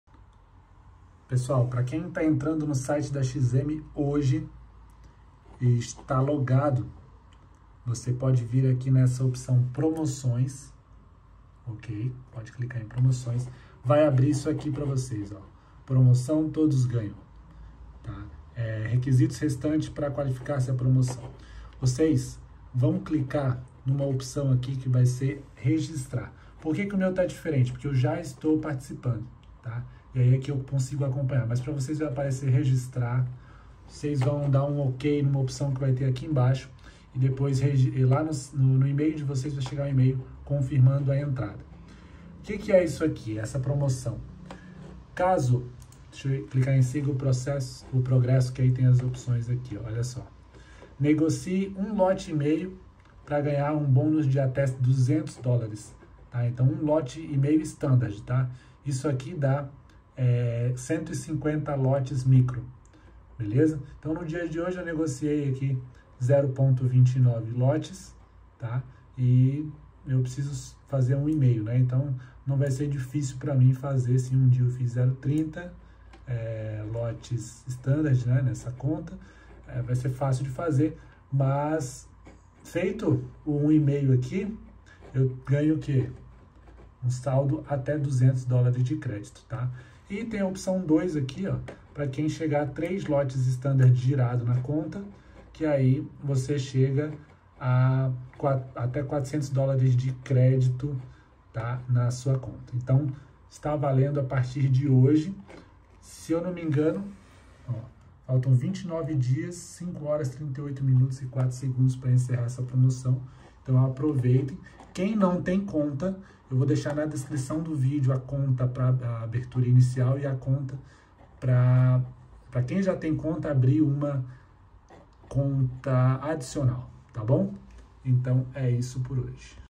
URGENTE - Promoção na XM - iniciada em 01/06/2022
Promoção iniciada na Xm onde voce pode ganhar até 400 usd

Abra uma conta pelo meu link ou crie uma conta adicional e participe